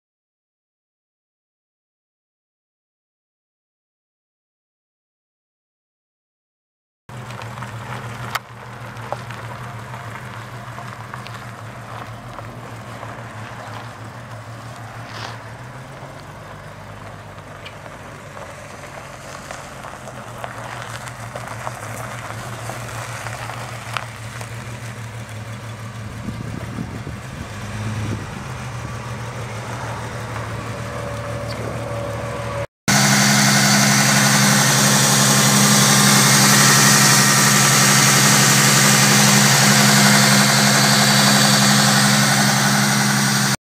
1995 Ford Mustang convertible for sale | sold at auction May 1, 2013
1995 Ford Mustang convertible
148,134 miles on odometer
3.8L V6 OHV 12V gas engine
Automatic transmission
AC and heat
Tilt
Cruise
AM/FM/CD
Power windows, locks, and mirrors
205/65R15 tires
1FALP4444SF230529

It was located in Rainbow, TX, and we sold it to the highest bidder for $1,705.